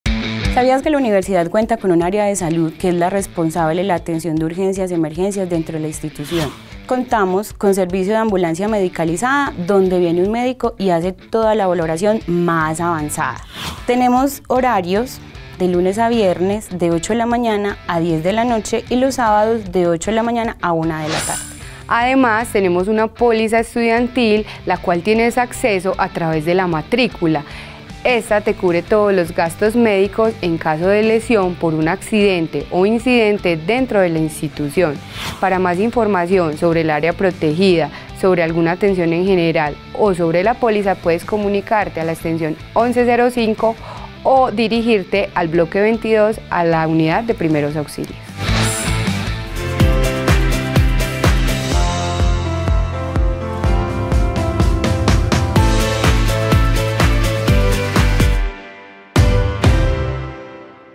VIDEO BIENESTAR
Sabías que la universidad cuenta con un área de salud que es la responsable la atención de urgencias emergencias dentro de la institución.